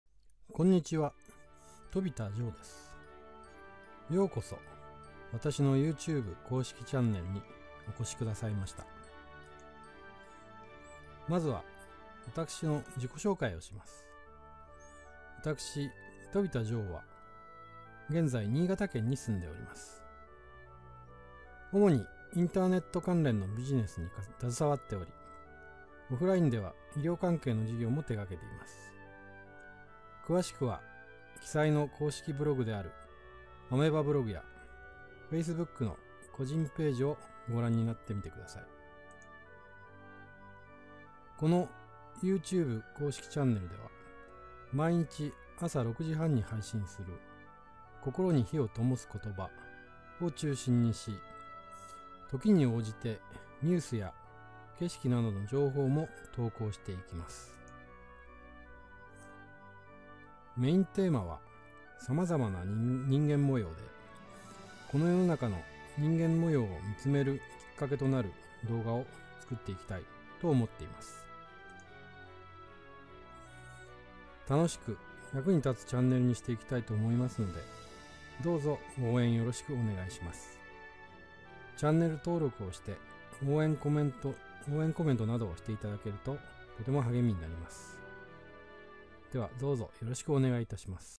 チャンネル紹介動画1
飛田丈公式チャンネル紹介動画です。
「様々な人間模様」をメインテーマに動画を投稿してゆきます。
どうぞ応援よろしくお願い致します。